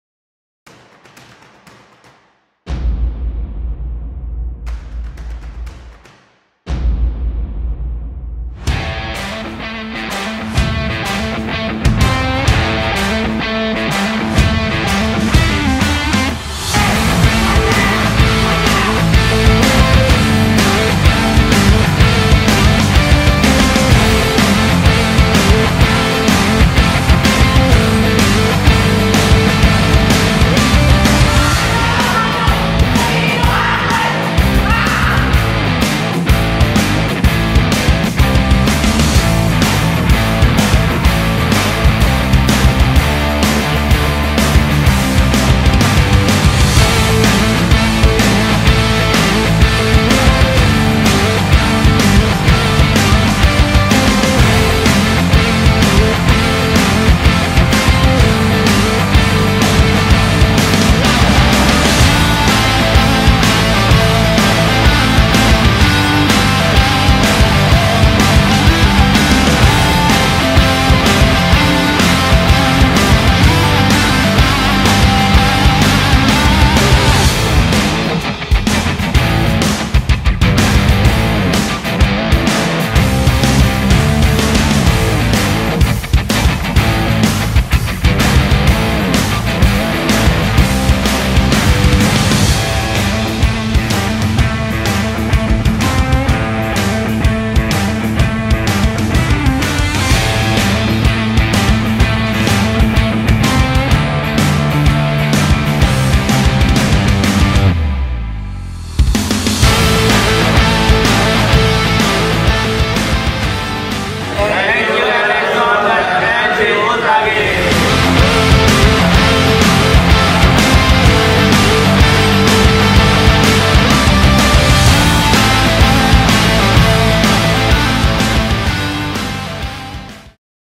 VPC SEASON 3 2023 AFTER MOVIE FINAL
VPC SEASON 3, 2023, owned the gaming world on December 2nd & 3rd. Big love to our main sponsors, LA ZONE and LG Ultragear. The next VPC is in the works – where Valorant communities connect. Stay in the loop for more details!